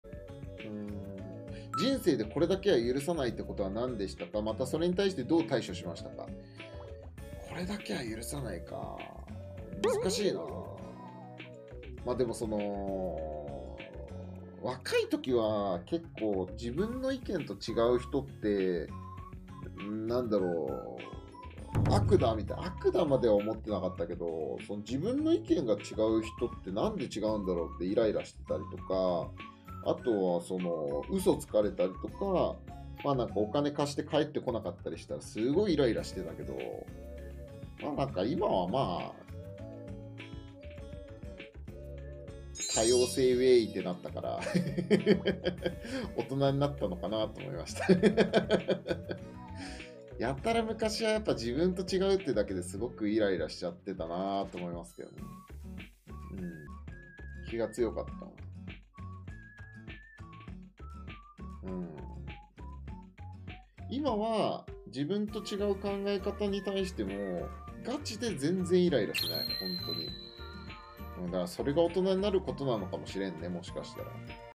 大人の成長！！ドズル社長の許せないことの対処法とは？【ドズル社切り抜き】【ドズル切り抜き】
ドズル社長は大人ですね...私とは大違いです笑
本チャンネルを見ていただきありがとうございます！

★ドズル社の皆さんのチャンネルはこちらです！